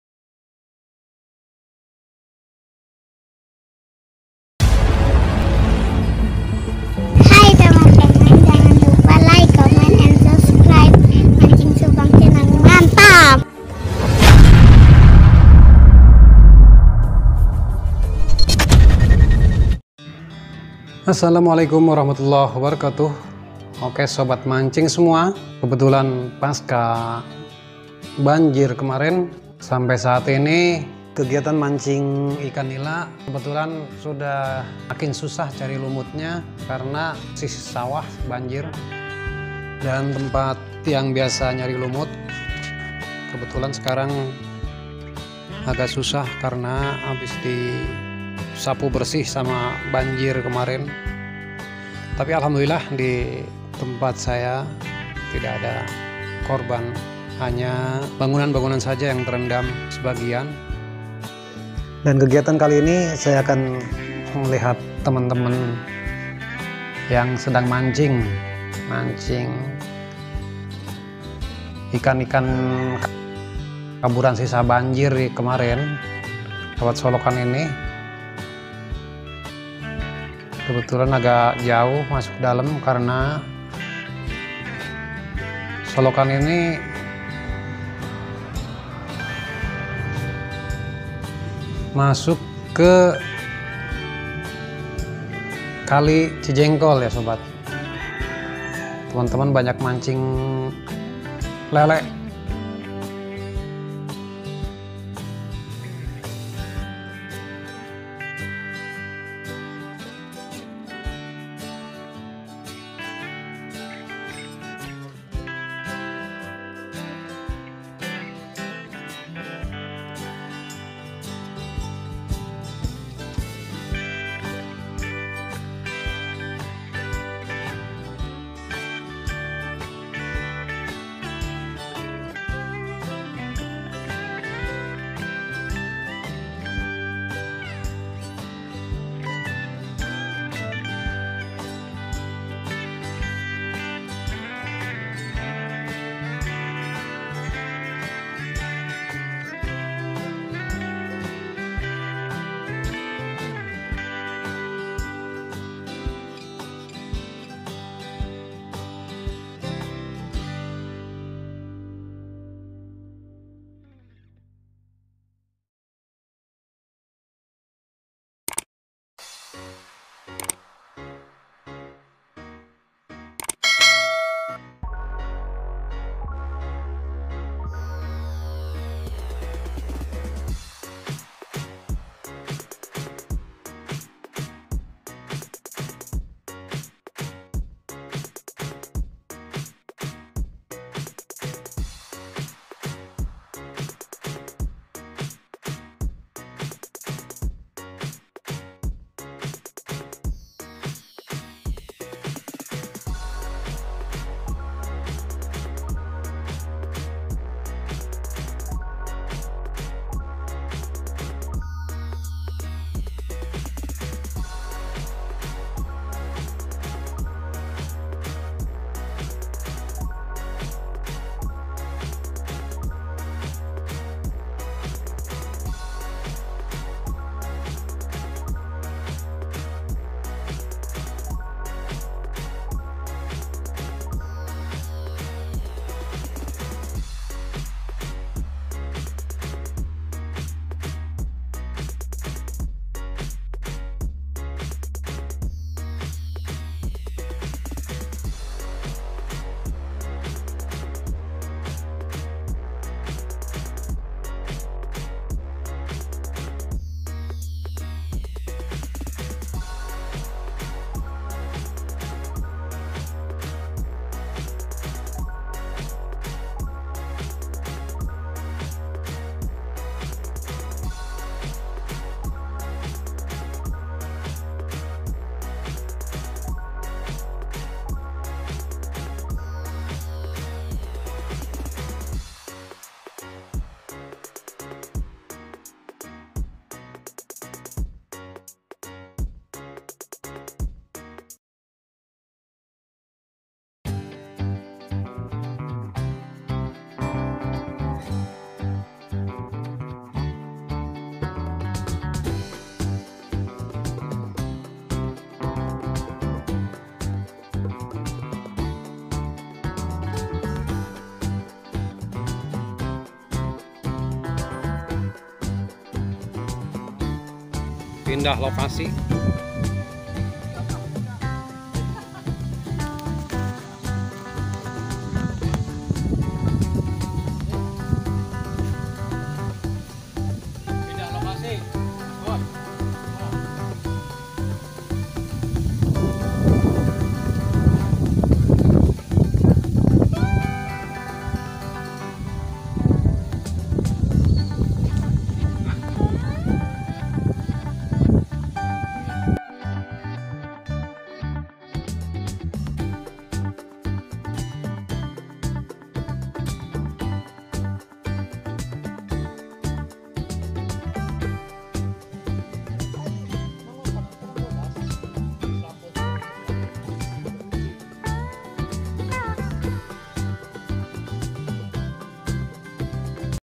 Amazing Fishing | mancing lele monster setelah banjir | mancing gaya gondrong labanan
Mancing ikan lele hasil kaburan setelah banjir surut, karena semua empang yang berdekatan dengan sungai banyak yang terendam banjir hingga semua ikan bebas bisa pergi kemana mana sesuai yang dia mau.